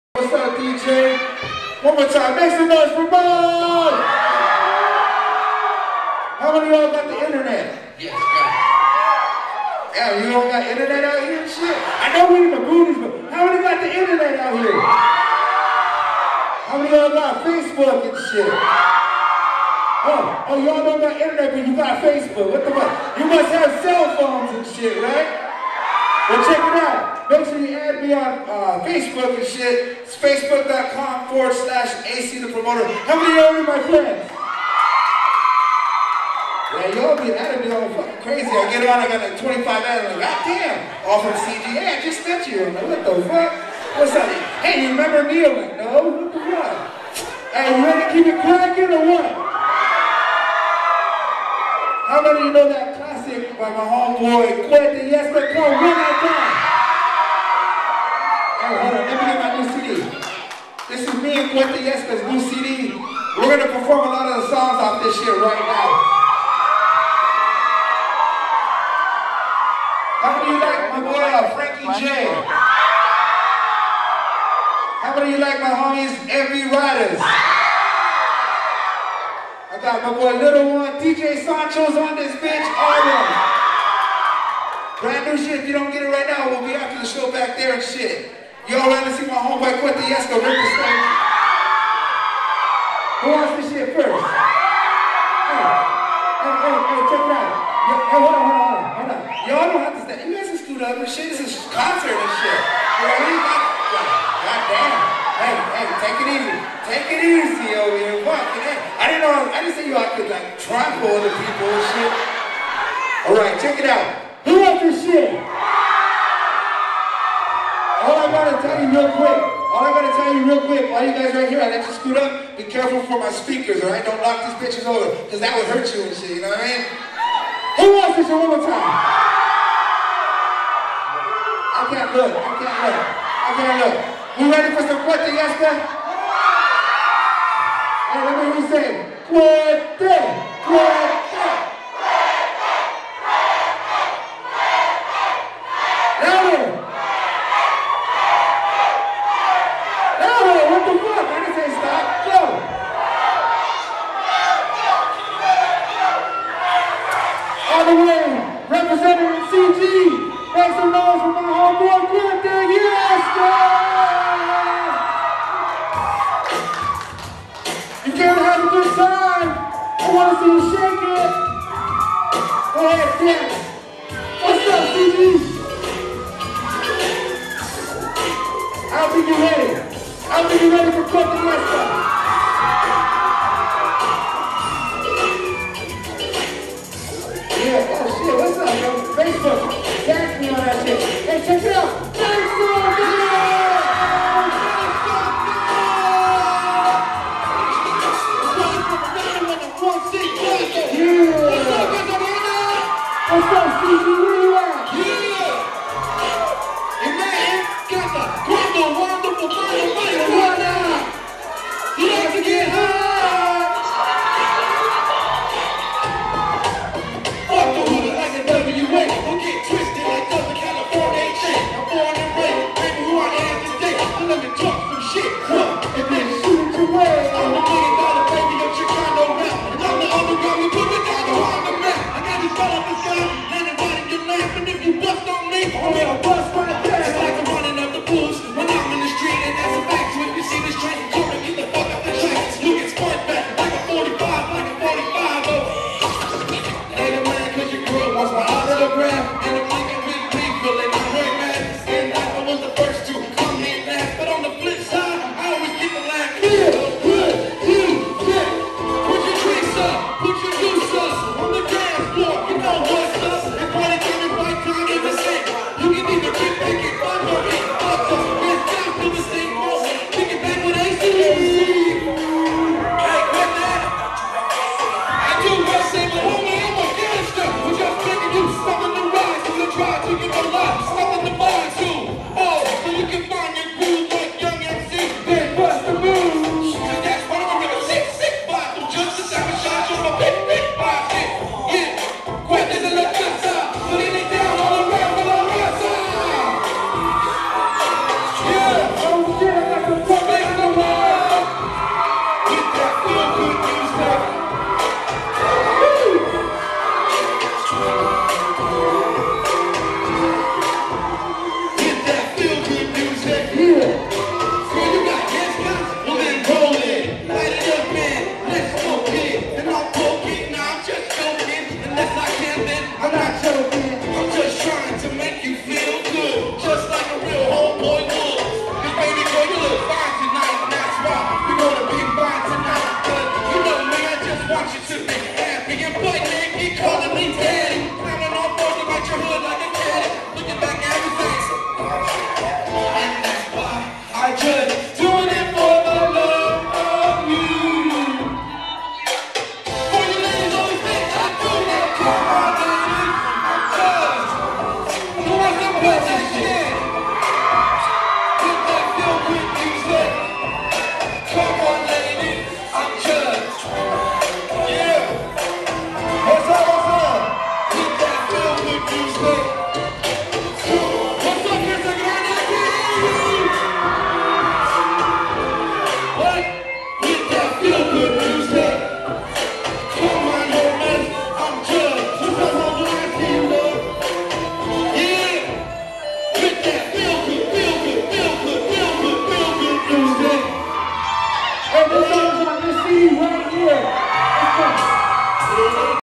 Mr. Capone-e, Cuete Yeska, Miss Lady Pinks, Mobs, HPG, Chicano Sureno Rap Videos
Concert featuring Mr. Capone-e, Cuete Yeska, Miss Lady Pinks, Mobs, & HPG.  Recorded on June 3, 2011 by Oldschoolcitylive.com, Oldschoolcity.com, Chicano Clothing, Lowrider Clothing, Lowrider Videos